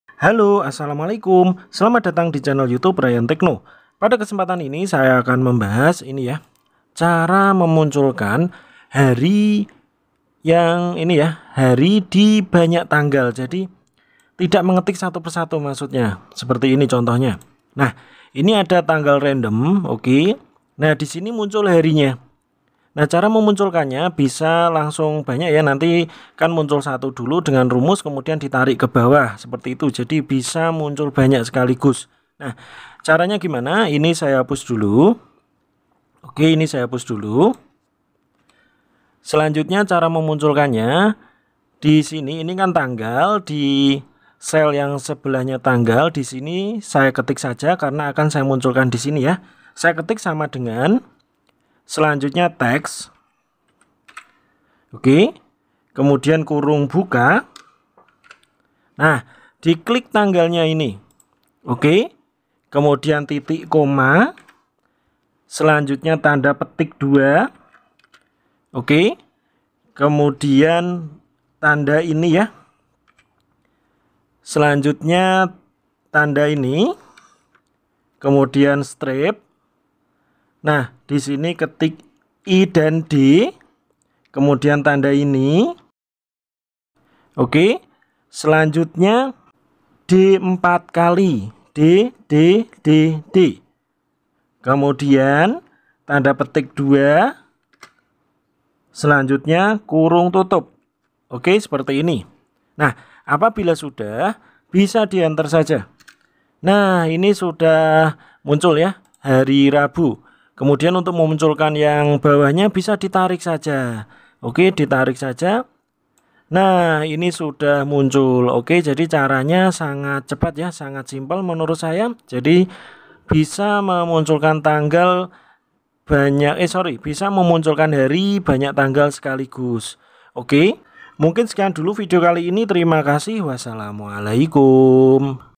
Cara Menampilkan Hari di Banyak Tanggal Sekaligus di Excel - Muncul di Cell Sebelah (Pakai Rumus)
Ini adalah cara menampilkan hari di banyak tanggal sekaligus di microsoft excel.